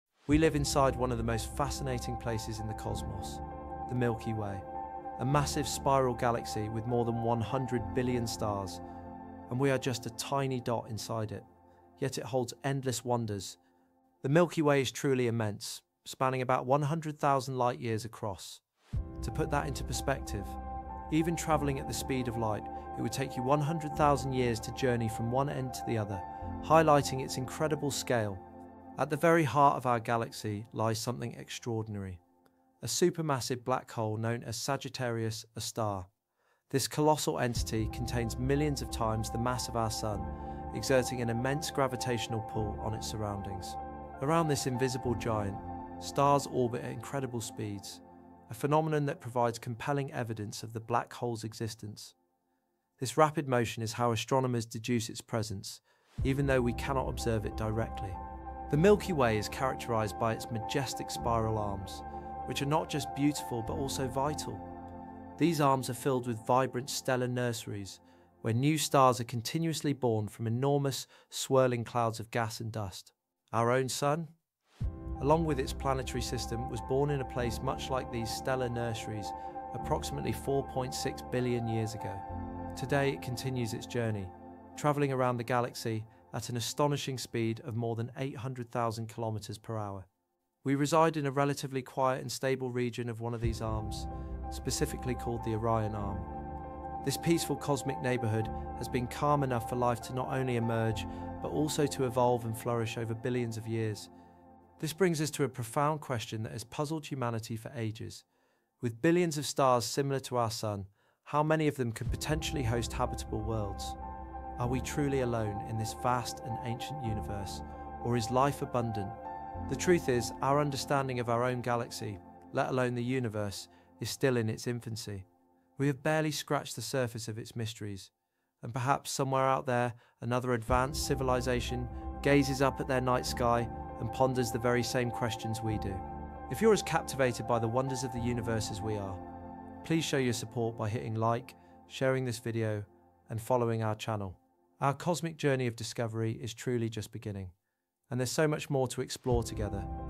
Milky Way: The Secrets of Our Home Galaxy 🌌✨
Discover the Milky Way like never before. Learn what's at the galactic center, where our Solar System is located, how stars are born, and why we might not be alone in the universe.